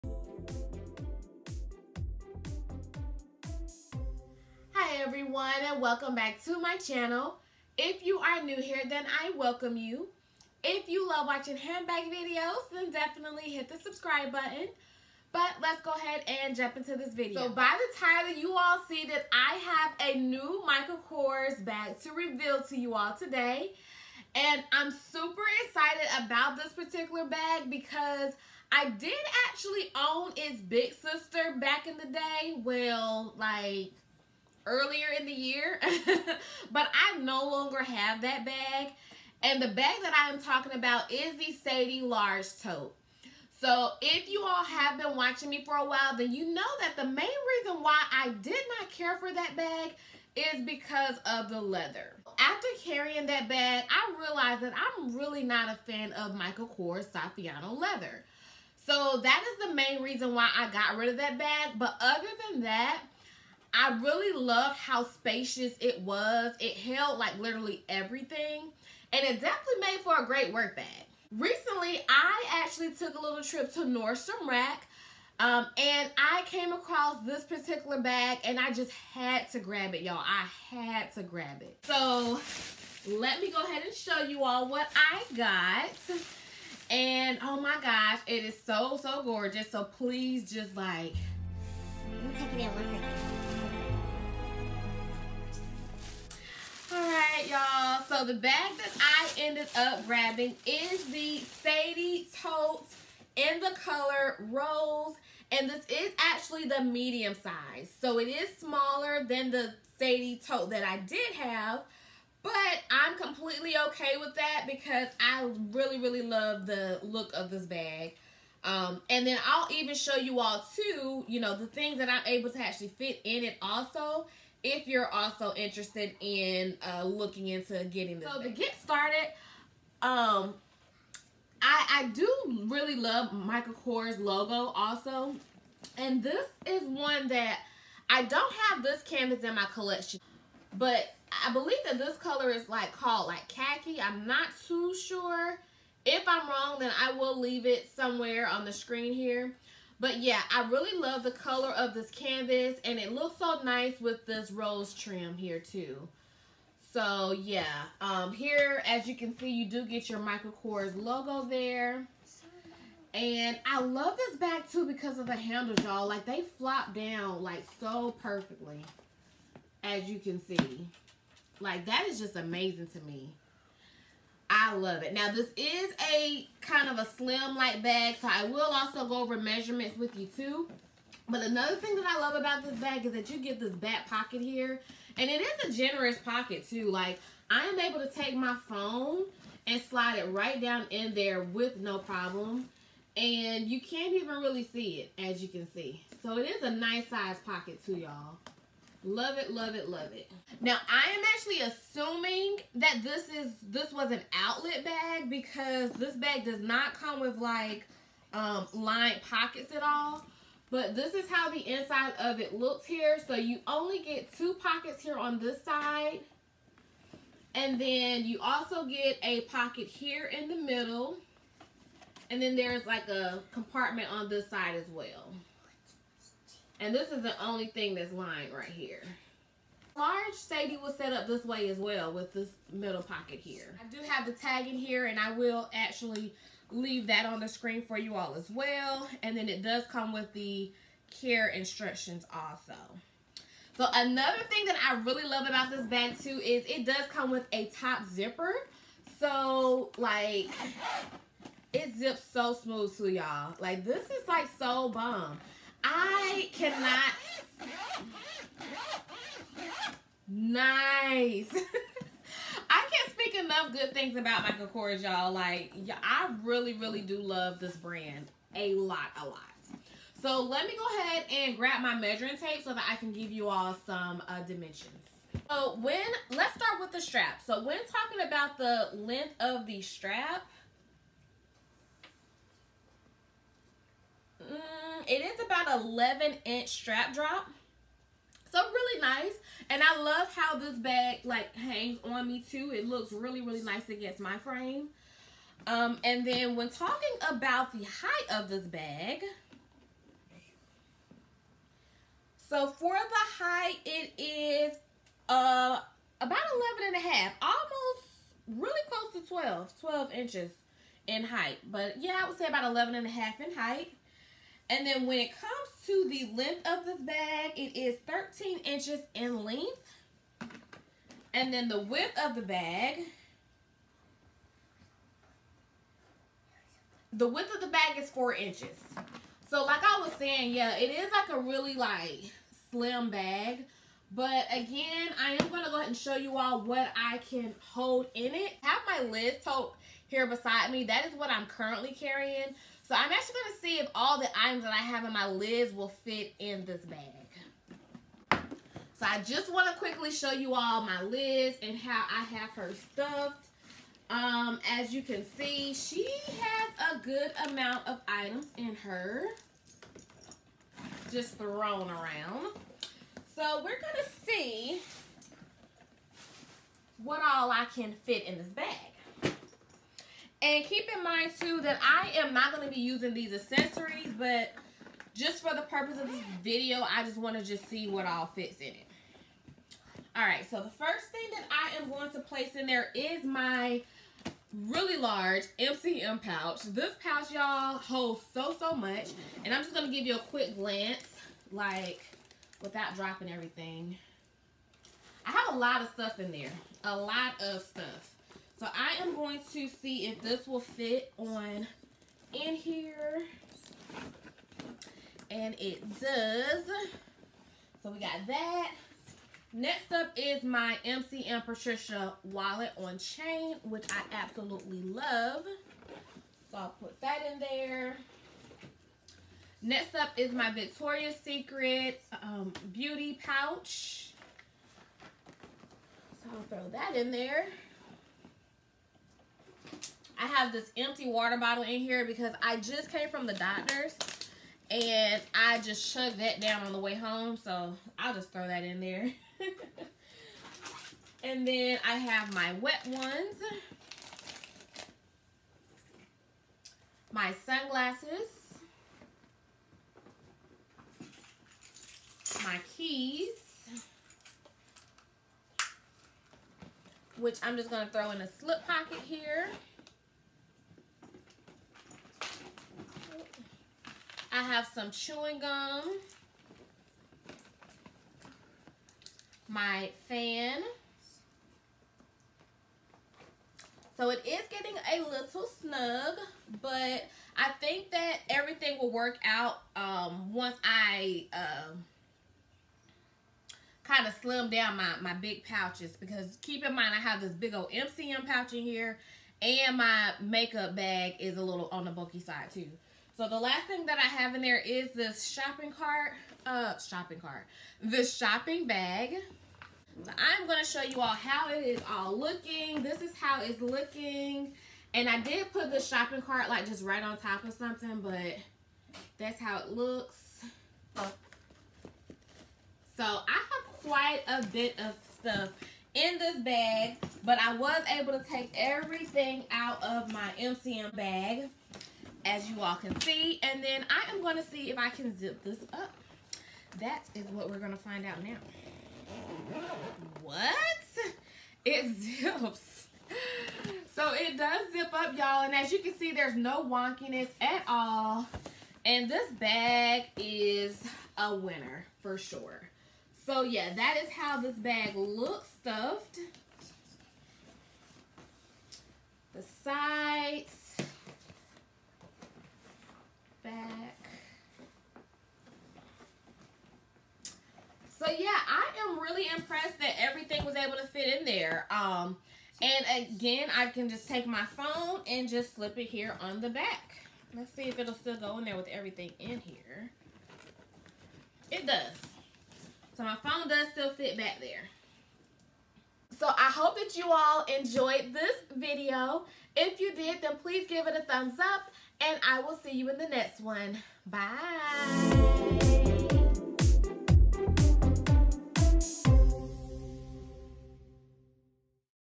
MICHAEL KORS SADY MEDIUM TOTE IN ROSE|REVEAL
Today I'll be revealing to you my Nordstrom Rack find!